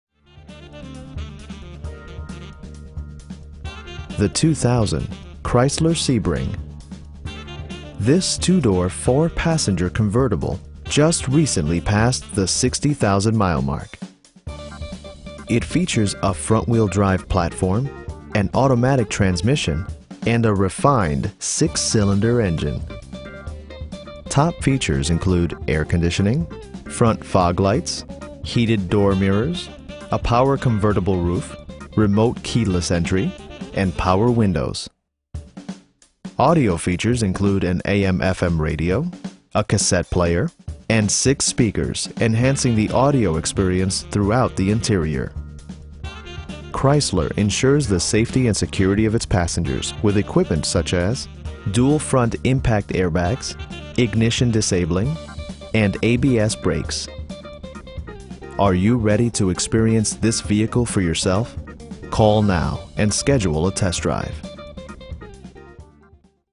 2000 Chrysler Sebring Convertible JXi
Jumbo Auto & Truck Plaza
1640 S. State Road 7,

The 2000 Chrysler Sebring. This 2 door, 4 passenger convertible just recently passed the 60,000 mile mark! It features a front-wheel-drive platform, an automatic transmission, and a refined 6 cylinder engine. Top features include air conditioning, front bucket seats, front fog lights, tilt steering wheel, heated door mirrors, a power convertible roof, remote keyless entry, and power windows. Audio features include an AM/FM radio, a cassette player, and 6 speakers, enhancing the audio experience throughout the interior. Chrysler ensures the safety and security of its passengers with equipment such as: dual front impact airbags, ignition disabling, and ABS brakes. Are you ready to experience this vehicle for yourself?